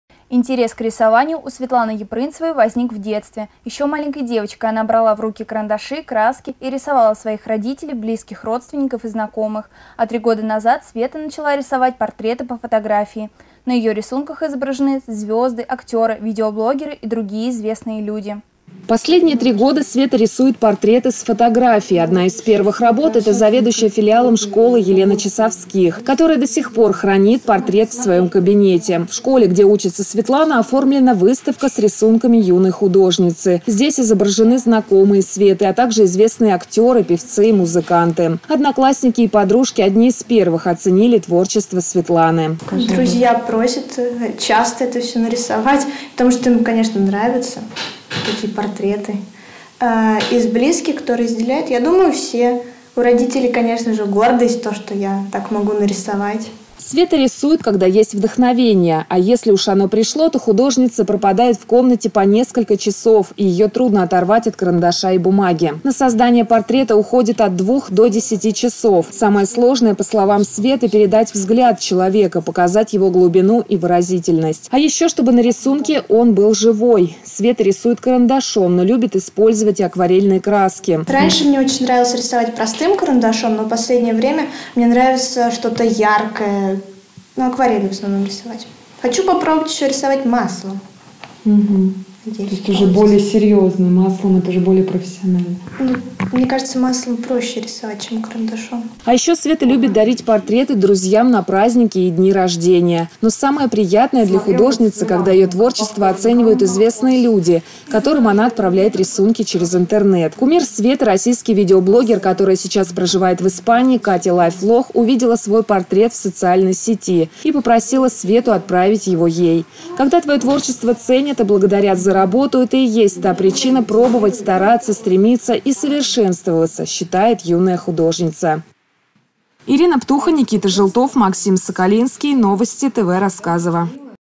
Когда приходит вдохновение
Интерес к рисованию у Светланы Япрынцевой возник в детстве.  Еще маленькой девочкой она брала в руки карандаши, краски и рисовала своих родителей, близких родственников, знакомых. А три года назад Света начала рисовать портреты по фотографии. На ее рисунках есть звезды, актеры, видеоблогеры и другие известные люди.
Ирина Птуха